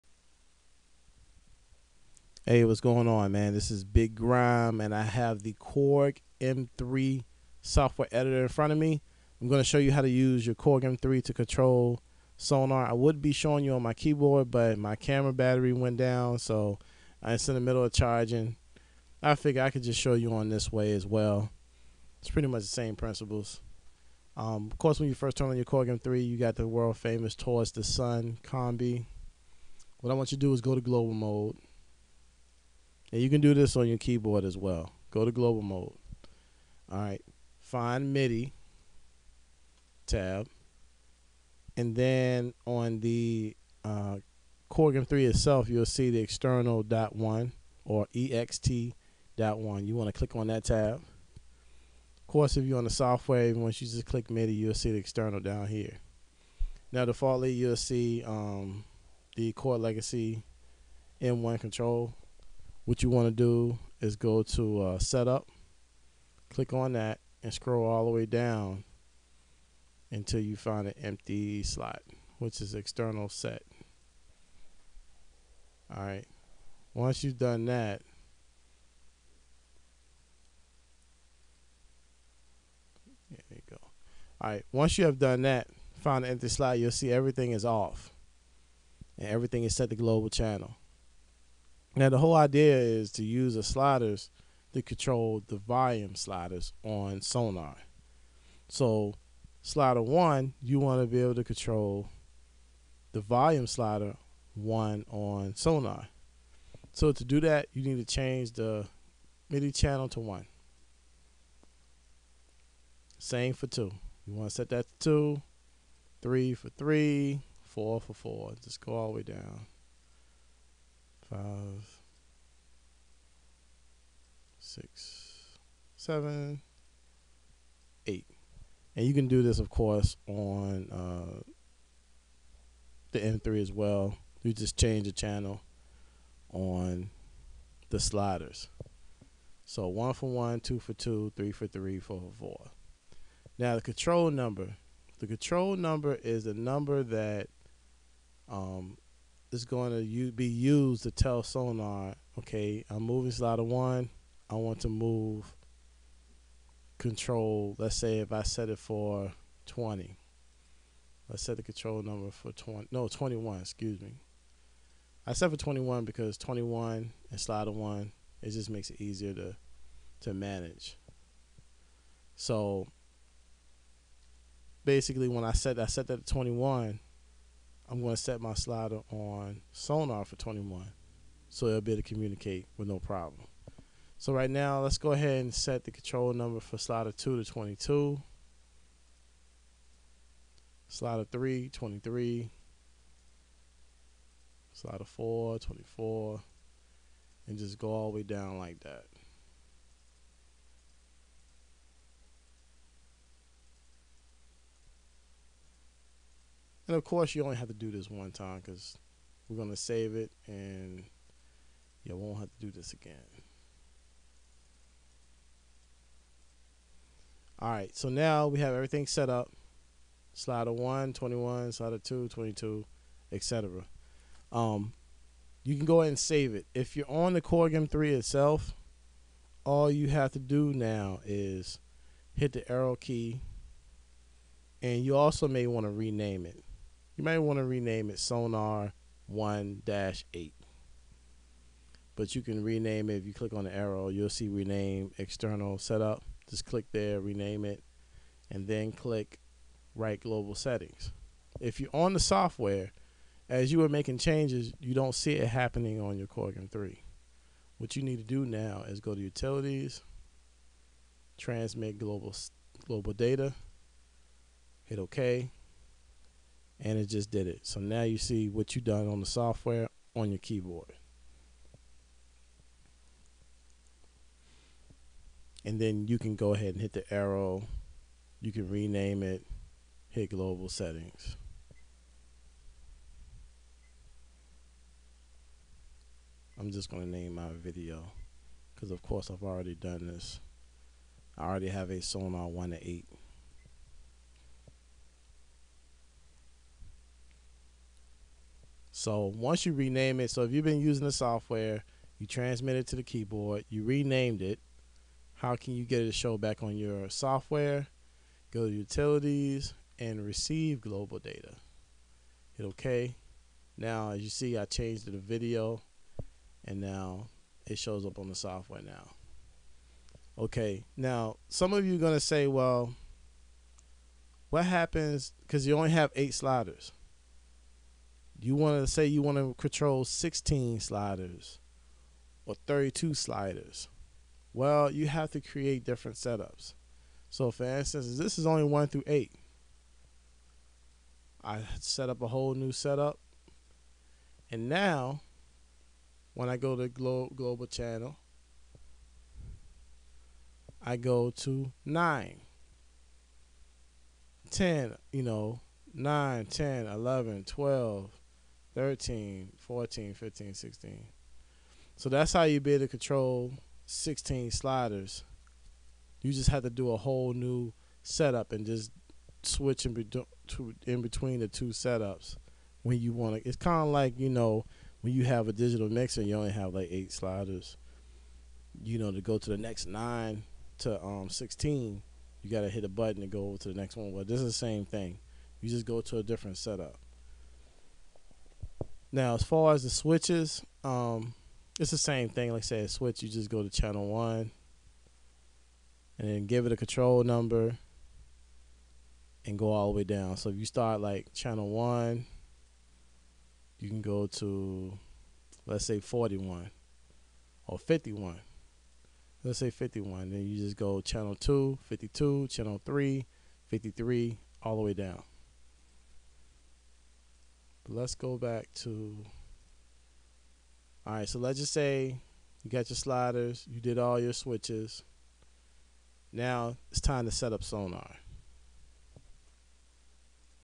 How to setup korg m3 to control sonar part 1
Set by set I show you how to setup you korg M3 and Cakewalk Sonar so you can mix using your sliders and buttons

thesixtyone